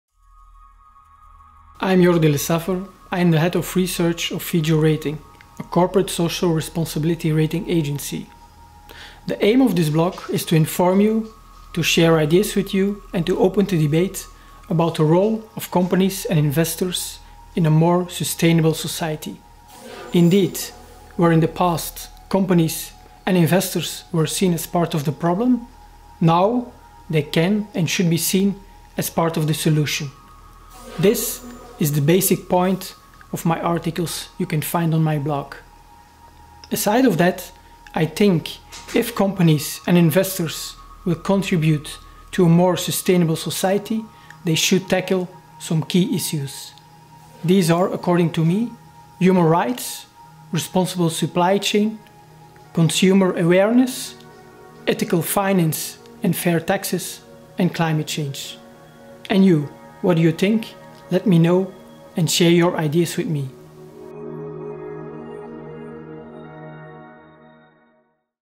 Jordi Lesaffer blog | ENG master
Thoughts and reflexions on a sustainable society.
Presentation of the blog of Jordi Lesaffer.

Een film van | Un film de | A film by

Sophie Spillemaeckers
Ludovic Schweitzer